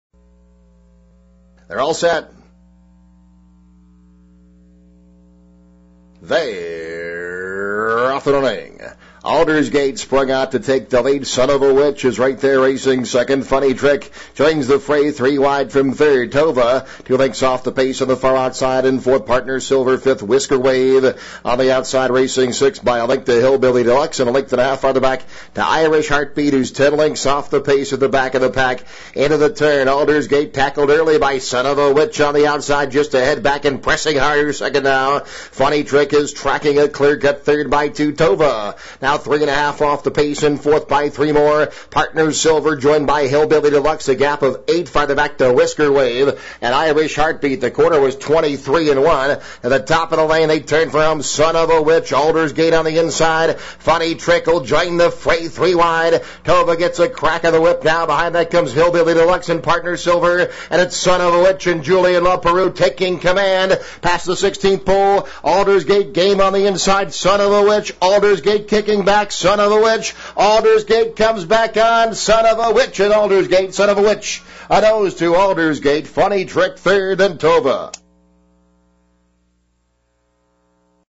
CHURCHILL DOWNS, 2008-04-27, Race 1
Entries: Thoroughbred Race, CHURCHILL DOWNS, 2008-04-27, Race 1:
      (Pgm-Runner-Jockey-Finish)
       1-Aldersgate (KY), Lanerie, C, finished 2;
       2-Sonofawitch (KY), Leparoux, J, finished 1;
       3-Hillbilly Deluxe (KY), Hebert, T, finished 5;
       4-Partners Silver (MD), Rodriguez, D, finished 6;
       5-Whiskerwave (KY), Castanon, J, finished 8;
       6-Irish Heartbeat (KY), Hernandez, Jr., B, finished 7;
       8-Funny Trick (KY), Borel, C, finished 3;
       9-Tovah (KY), Lopez, J, finished 4;